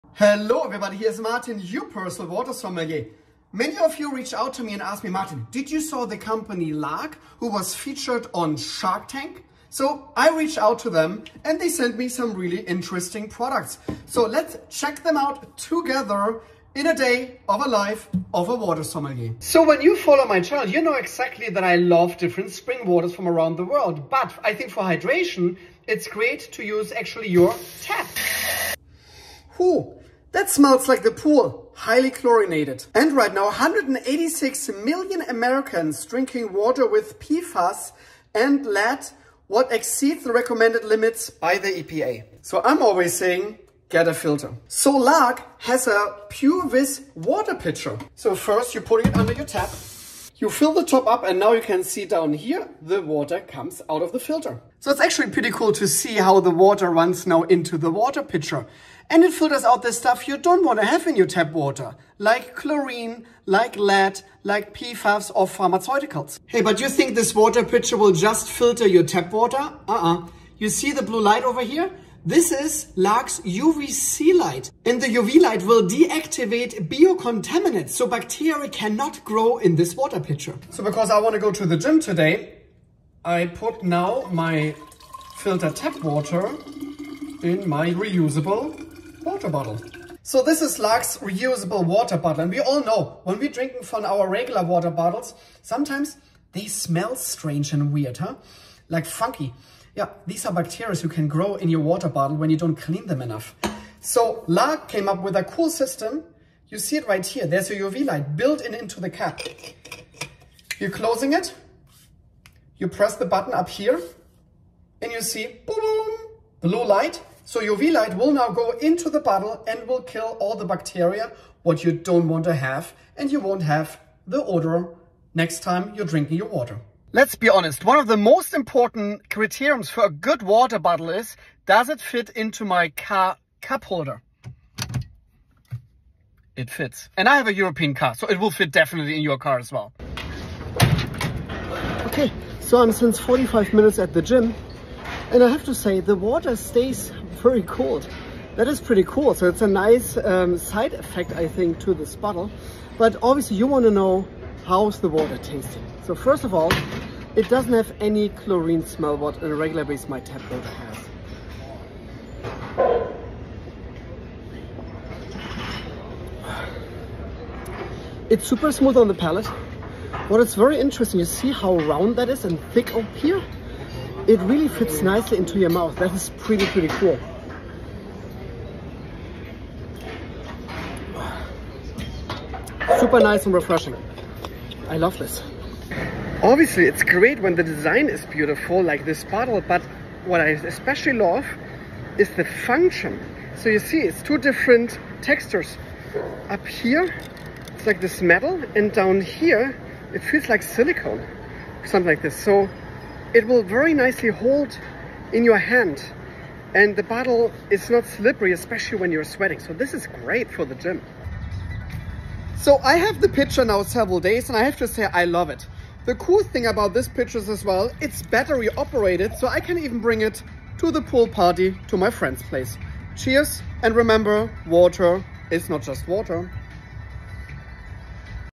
LARQ Bottle & Pitcher PureVis™ reviewed by Martin Riese Water Sommelier
LARQ send me over the self cleaning water bottle & pitcher PureVis™ so I checked it out and here is my review.